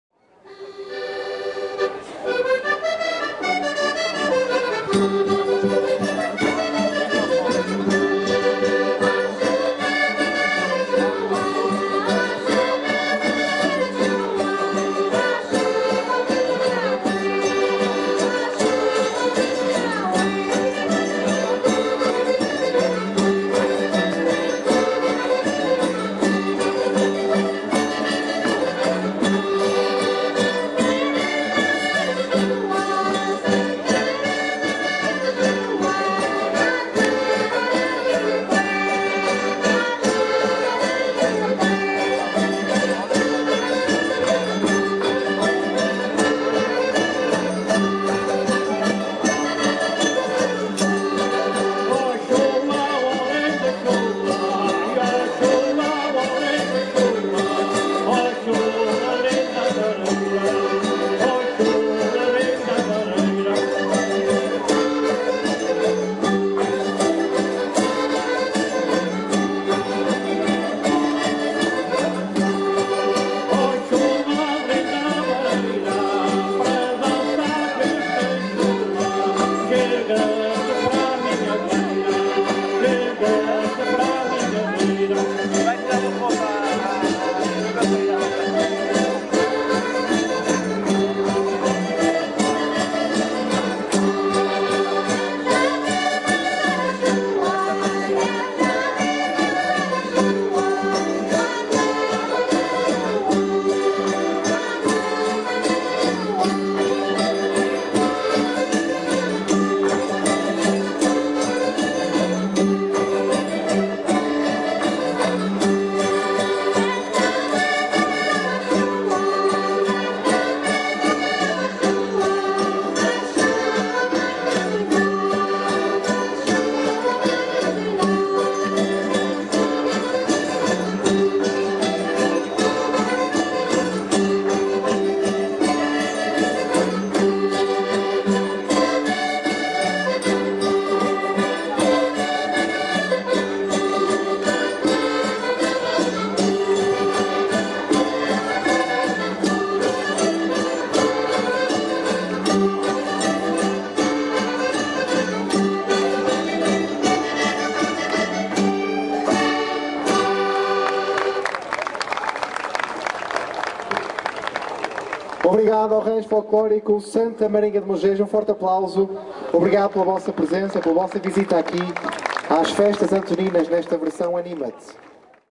"Chula" - Rancho Folclórico Santa Marinha de Mogege, Vila Nova de Famalicão.
10 de Junho 2021 -  Festas Antoninas 2021.  Actuação do Rancho Folclórico Santa Marinha de Mogege, Vila Nova de Famalicão, Braga, Minho, Portugal.
Folclore ; música popular.